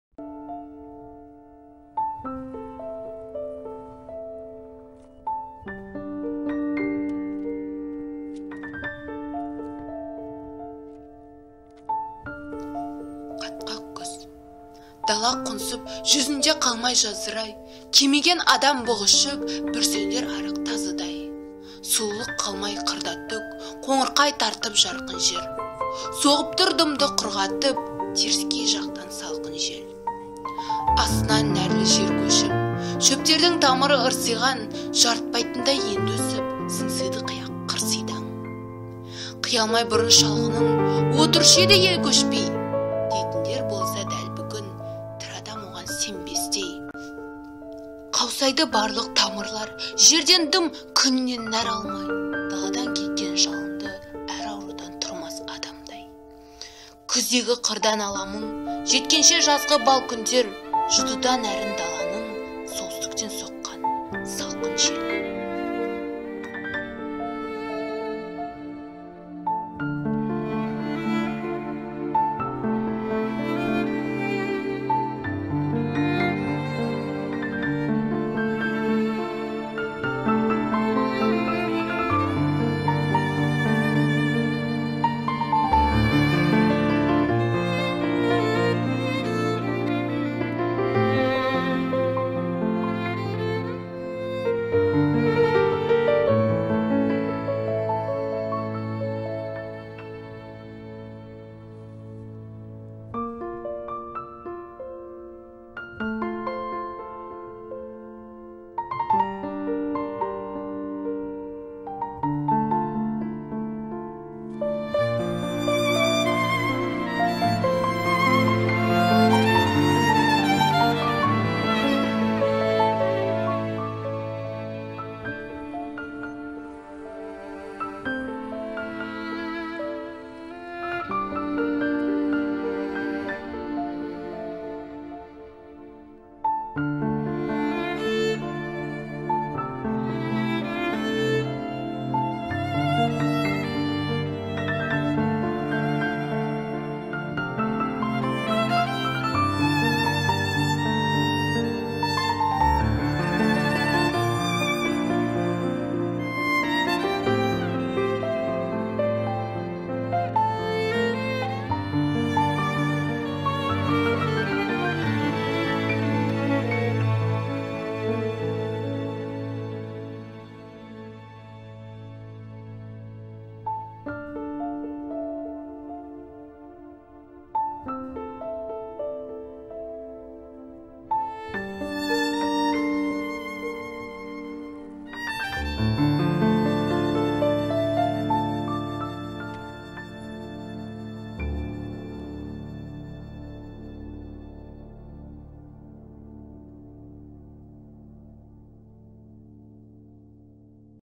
Ф. Оңғарсынова "Күз". Өлеңді оқыған: Сапарова Мәдина
Ф. Оңғарсынова "Күз". Өлеңді оқыған: Сапарова Мәдина №53 мектеп-лицейінің 11 сынып оқушысы.